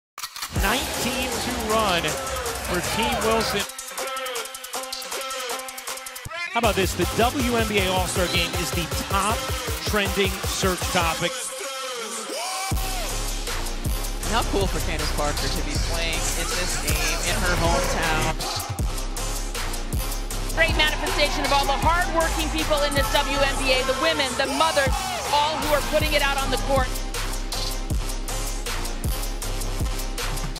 WNBA All-Star Returns To Las Vegas For 2023
The AT&T WNBA All-Star Game makes its' return to Las Vegas on July 15th 2023